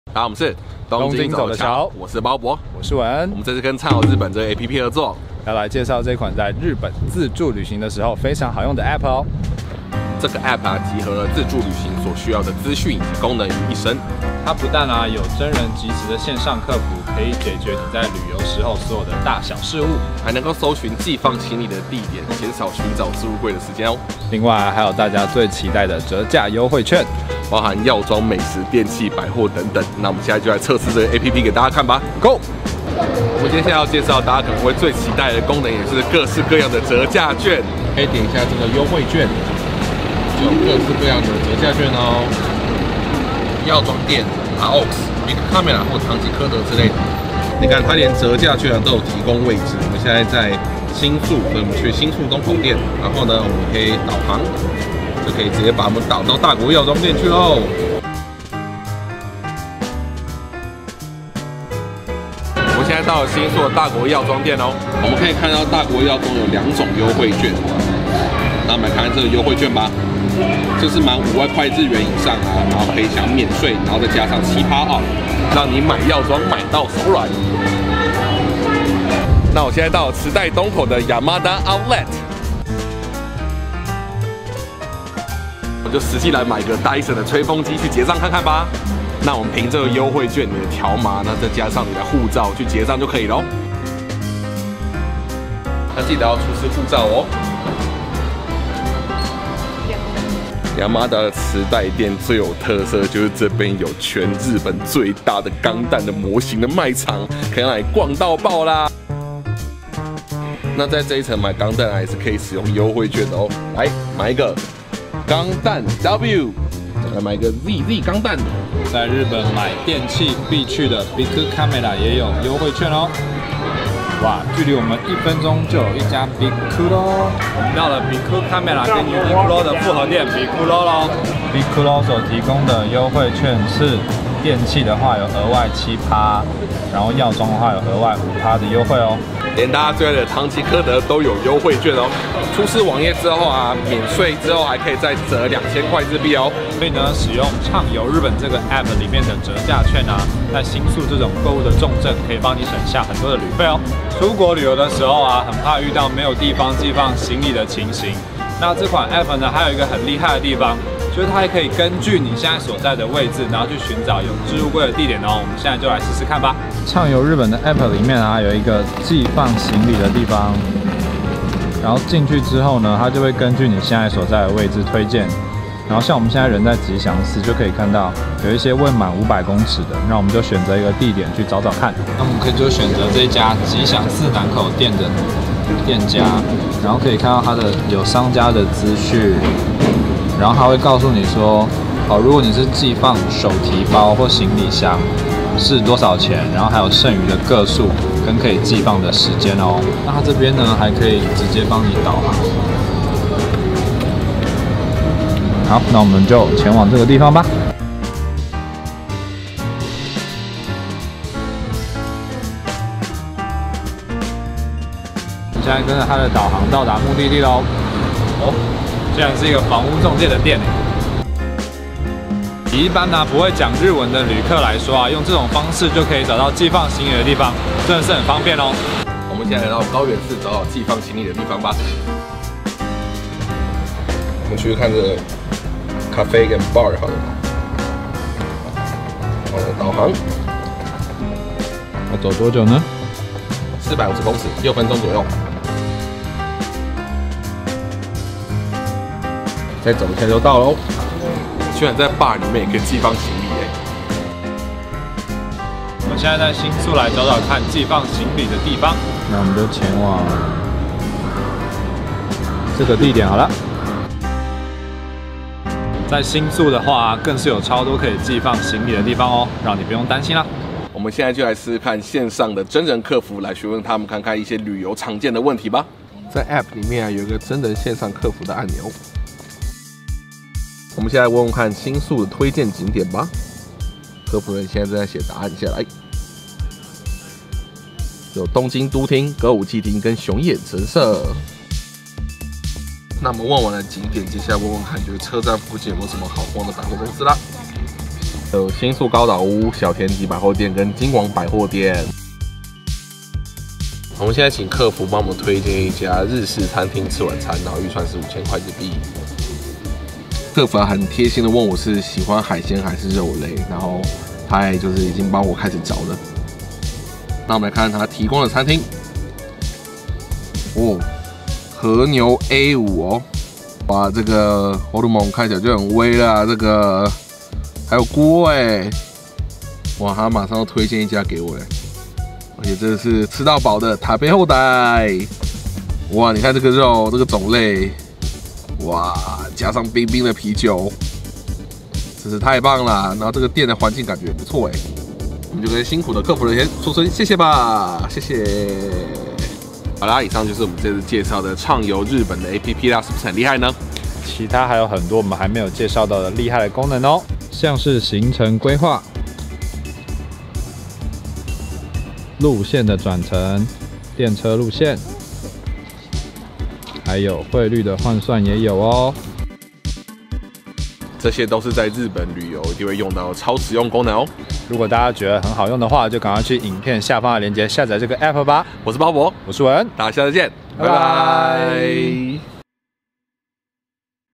日本旅遊必備APP推薦｜日本自由行必看
日本旅遊的時候，總是覺得很多小事情不知道問誰？
置物櫃一下就滿了，總是找不到地方放行李？
藥妝店買了一堆東西，退稅後還想要再便宜一點？
如果你有以上的困擾，那你一定要看看東瞧推薦的這款APP「暢遊日本」
他不但有真人線上客服、行李寄放查詢還有各種熱門商店的優惠券集合，絕對是日本旅行必備良藥！

下載連結

暢遊日本APP，搞定旅遊大小事
・ 線上真人客服
・ 商品翻譯
・ 行李寄存
・ 電車轉乘
・ 匯率換算
・ 優惠券大集合
・ 行程規劃...etc